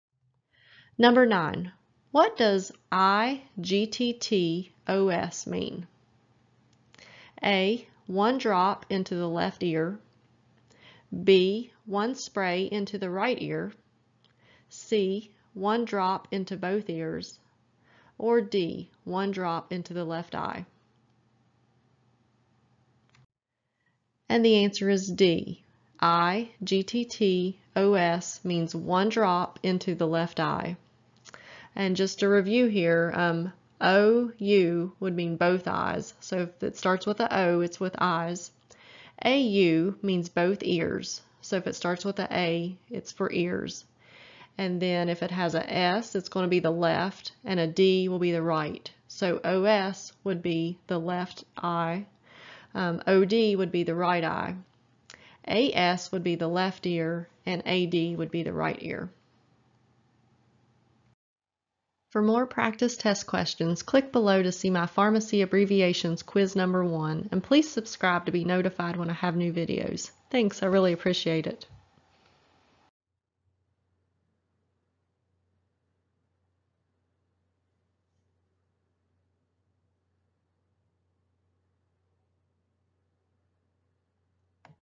Pharmacy Abbreviations and Sig Codes PTCB Test Prep Question - What does "i gtt os" mean?
Pharmacy Abbreviations and Sig Codes PTCB Test Prep Question 9 - What does "i gtt os" mean? Individual pharmacy sig code quiz questions from my Pharmacy Abbreviations Quiz #1 Video.